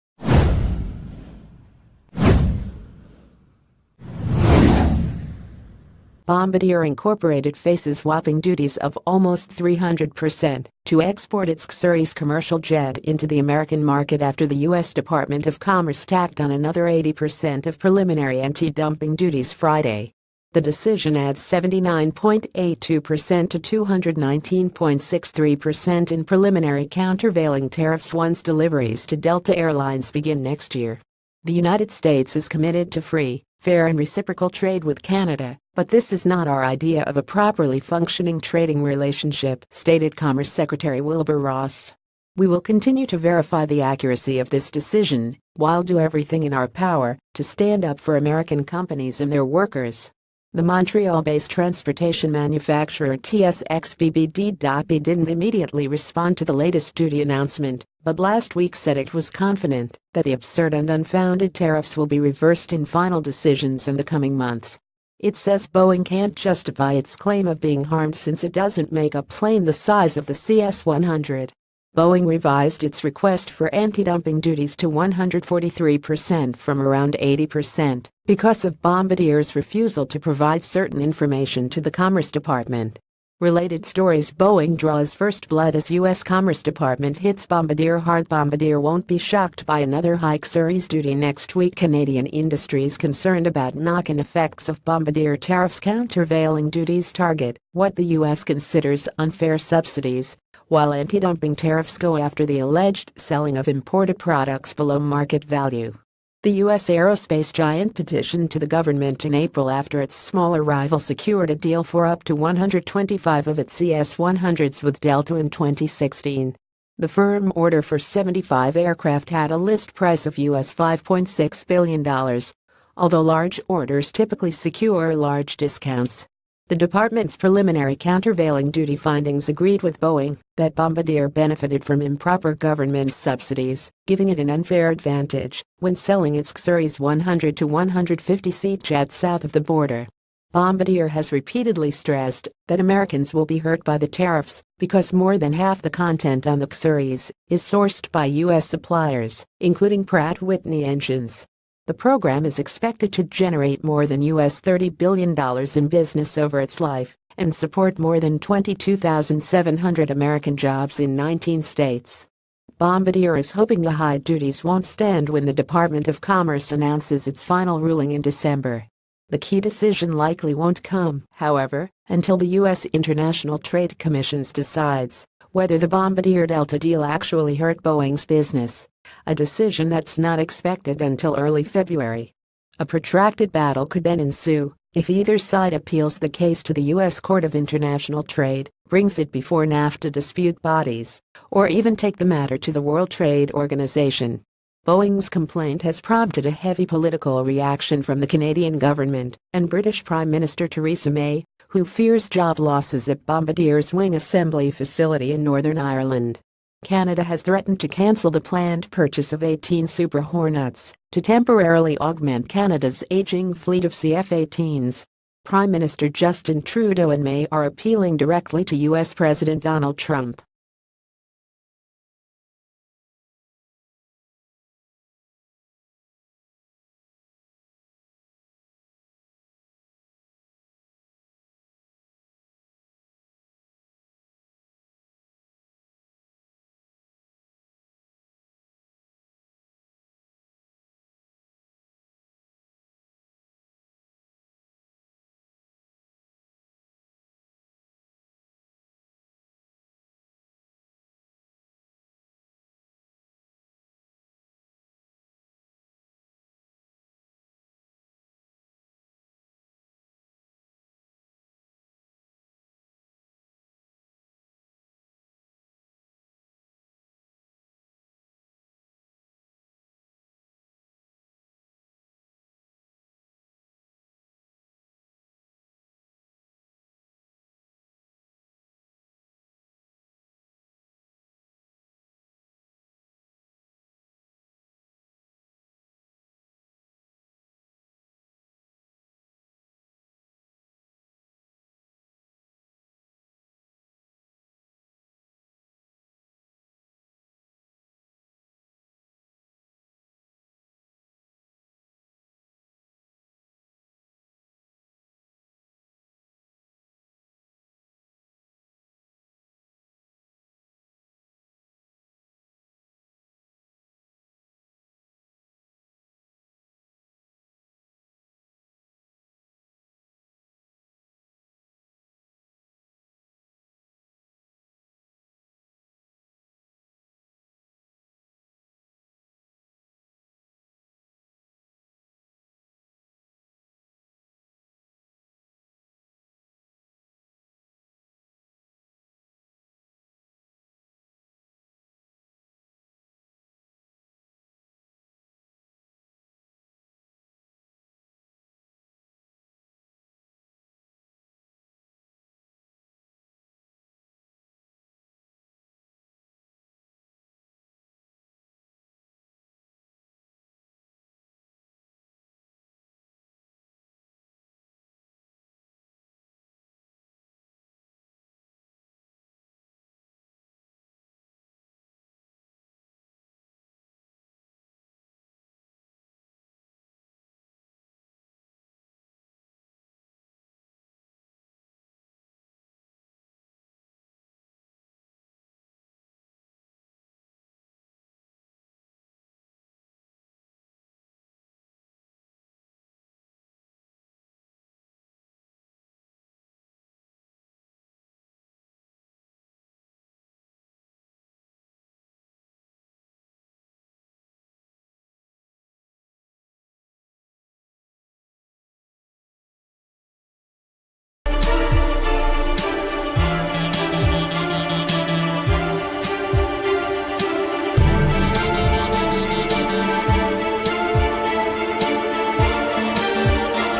Breaking News | U.s. adds another 80% in duties on bombardier cseries exports - citynews toronto
U.s. adds another 80% in duties on bombardier cseries exports - citynews toronto

Bombardier Inc. faces whopping duties of almost 300 per cent to export its CSeries commercial jet in...